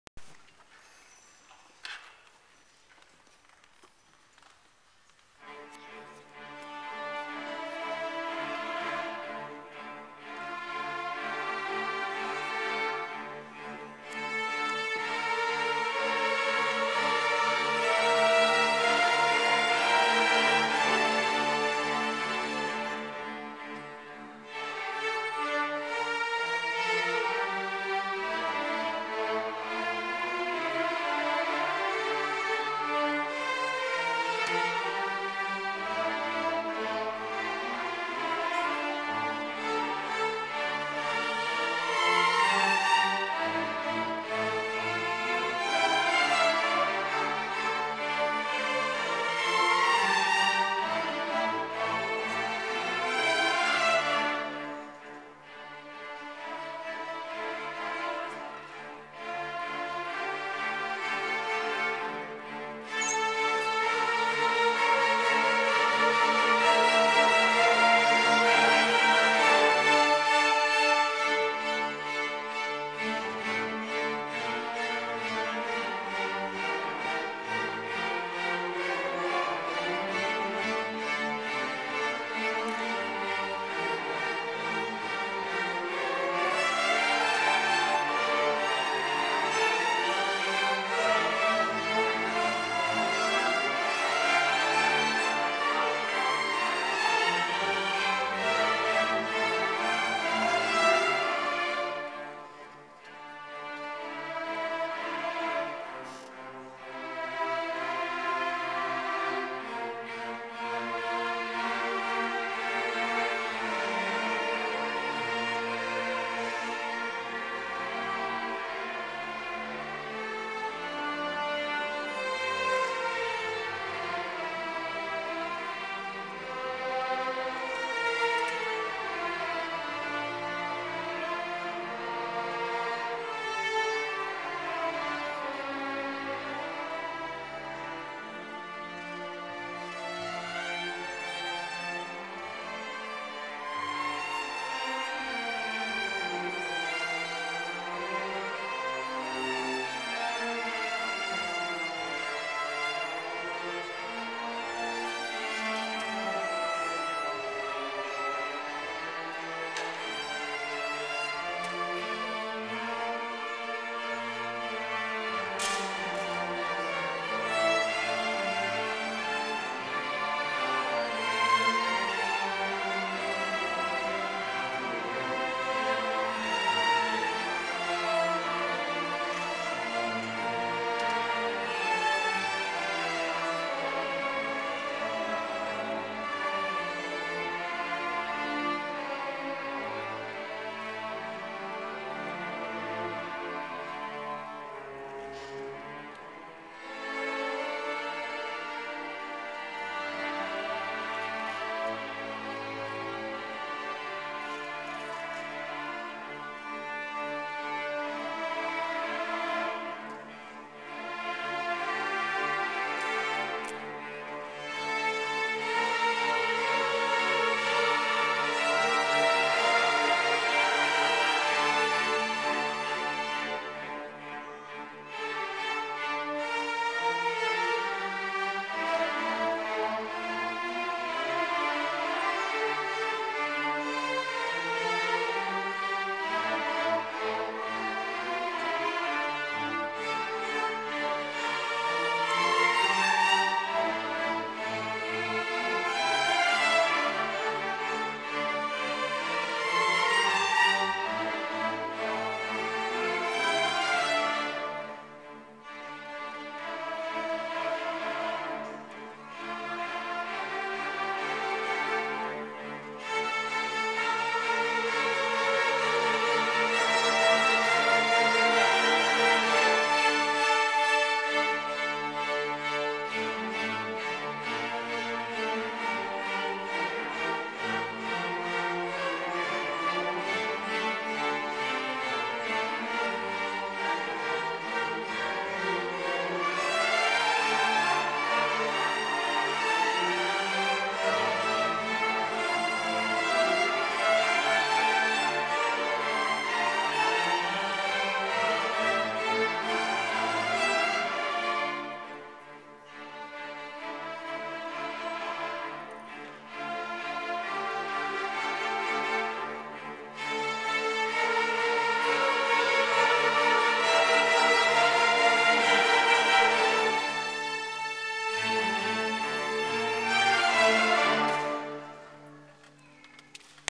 Oregon Suzuki Camp Orchestra B Performing recital on 6/29/2012
Sophie, Shannon and Iris perform!